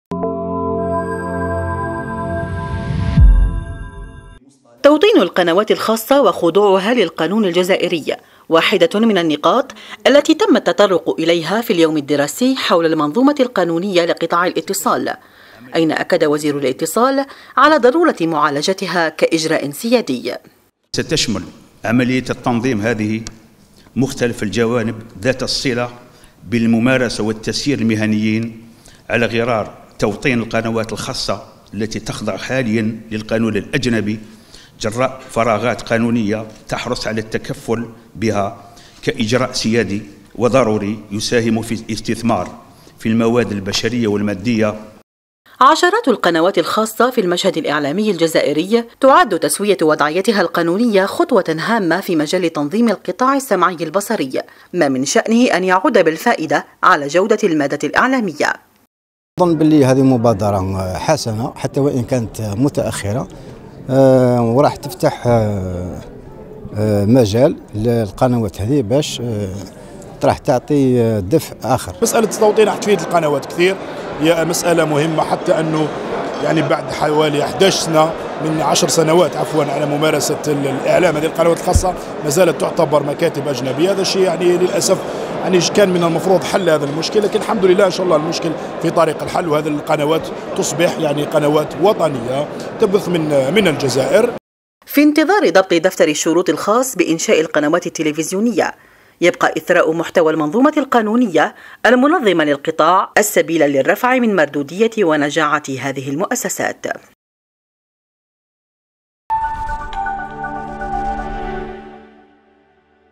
توطين القنوات الخاصة ..ضرورة للرفع من جودة المادة الإعلامية في الجزائر
الجزائر (وأج) - توطين القنوات الخاصة ..ضرورة للرفع من جودة المادة الإعلامية في الجزائر